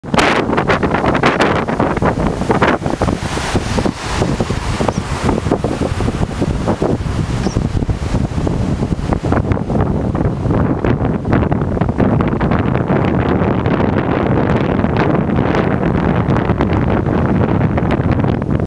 Cuttings Beach tide of pollution a cause of concern
SHOCKED, appalled and disgusted. This is the impenitent description of what has become of Cuttings Beach in Merebank by a community member, Levesh Preethrajh who was completely appalled at the state of the beach upon a recent visit.
Ward 68 councillor, Aubrey Snyman said that the pollution at Cuttings Beach has been an ongoing problem for years.